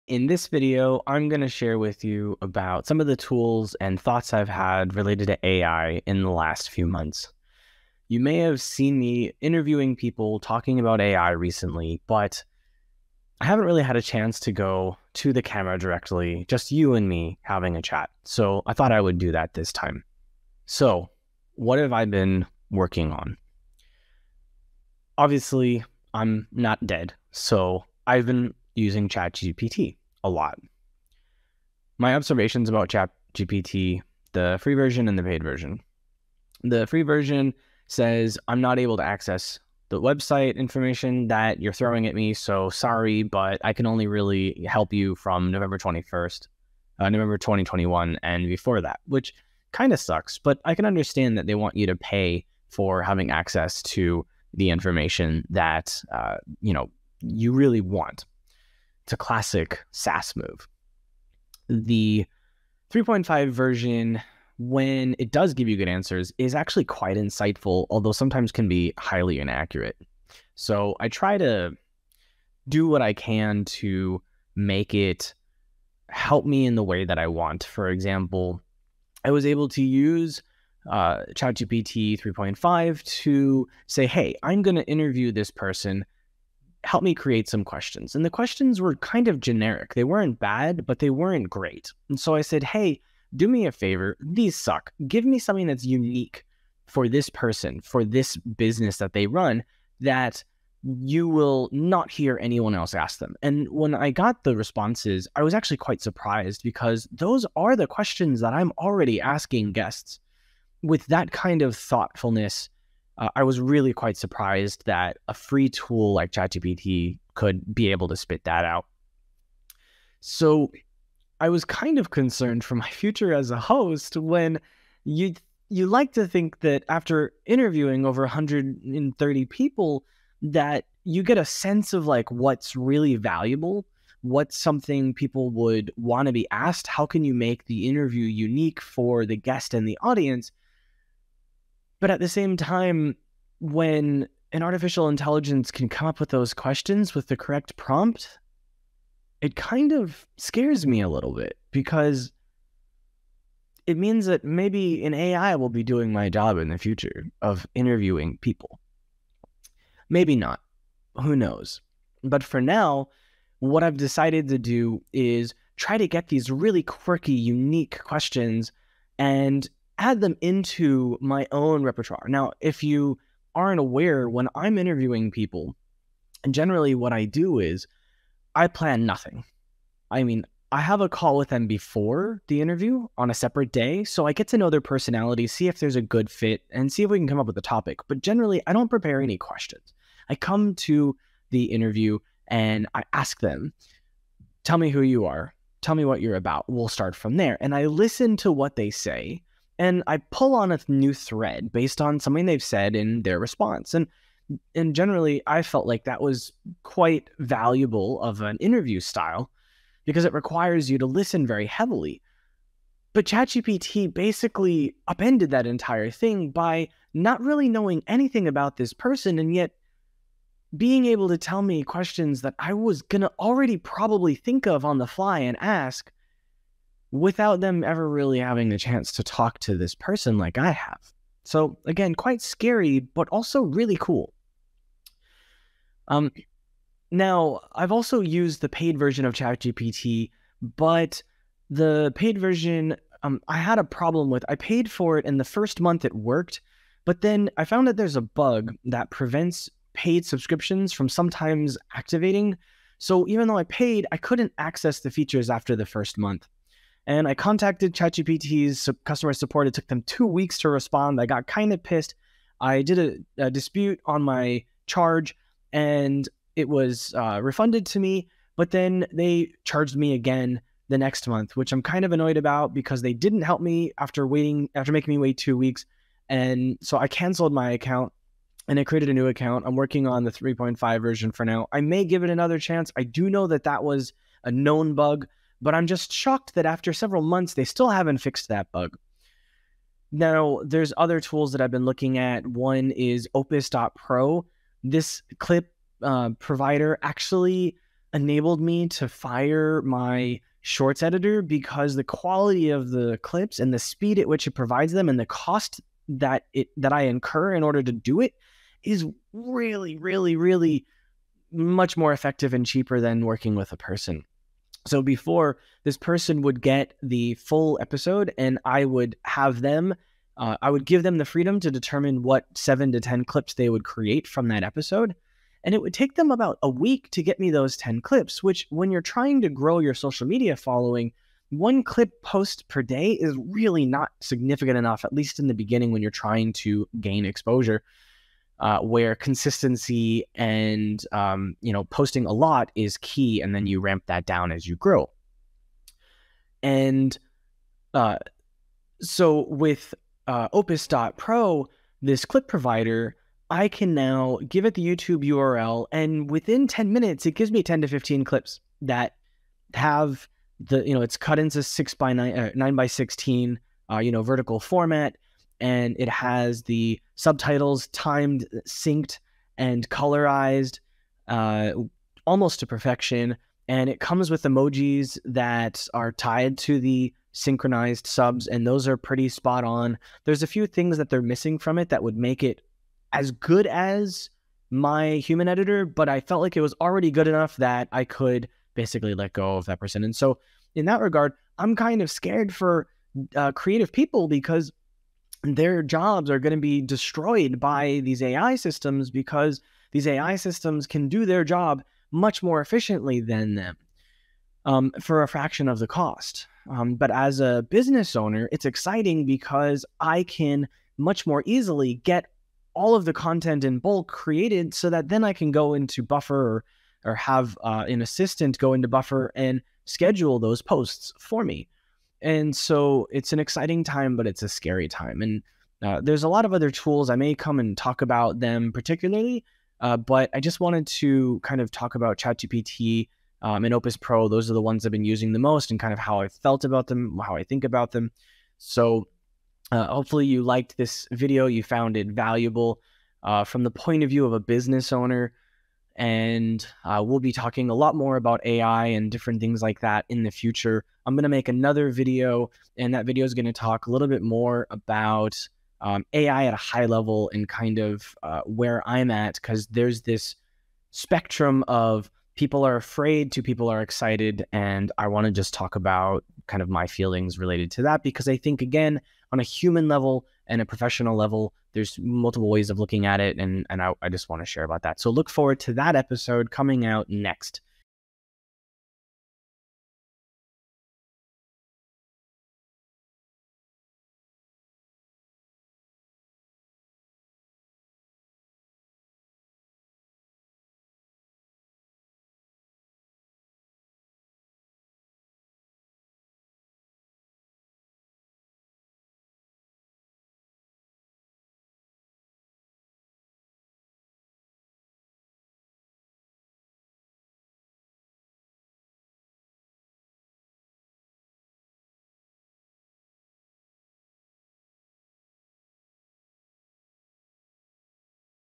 Should you be scared of ChatGPT?
I share my experience with AI tools, particularly ChatGPT and Opus Pro. The potential impact of AI on creative jobs and express their excitement as a business owner to easily generate content.

00:00 - Thoughts on Using AI for Interviewing and the Future of the Industry
03:20 - The Value of AI Interviewing and Clip Providers
06:38 - The Excitement and Fear of AI for Business Owners